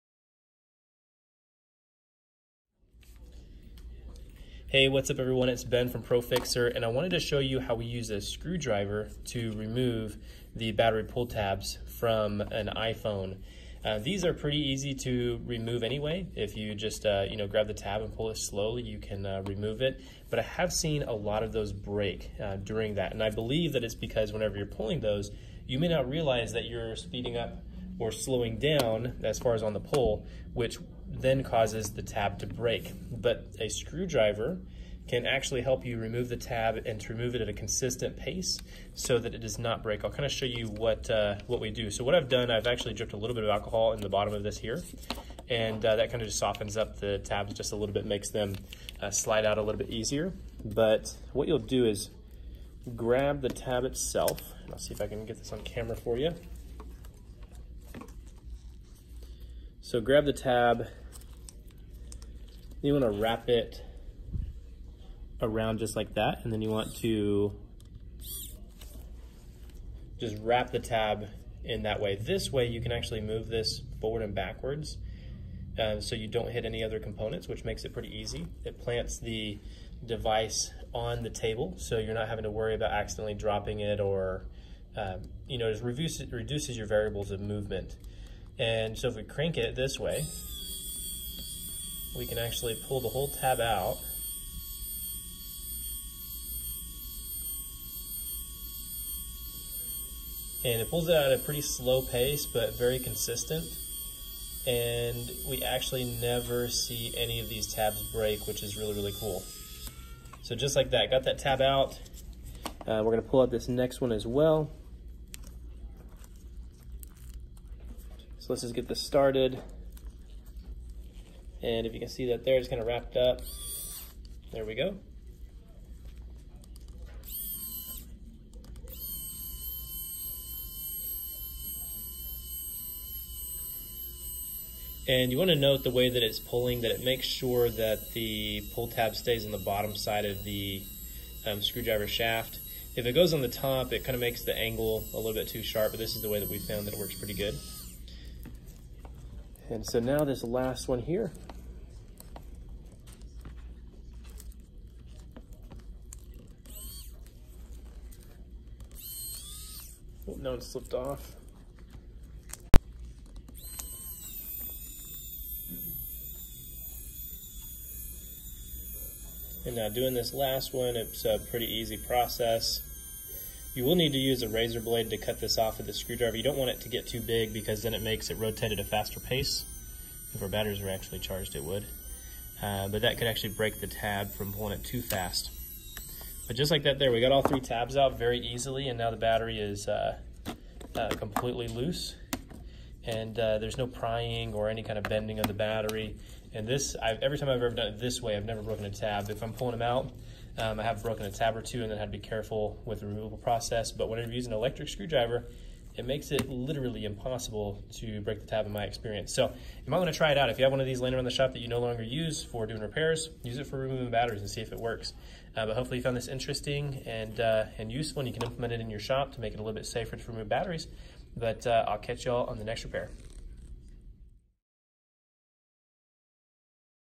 How to easily remove an iPhone battery with an electric screwdriver! Easy Method!
The battery pull tabs on iPhone batteries are amazing...when they work. Most of the time they come out fine, but when they dont it makes for a very difficult and delicate battery removal.

By using an electric screwdriver, the battery pull tab experiences less stress and comes out easily.

iFixit Pro Tech Toolkit

XOOL 80 Piece Tool Set

MakerHawk USB Ammeter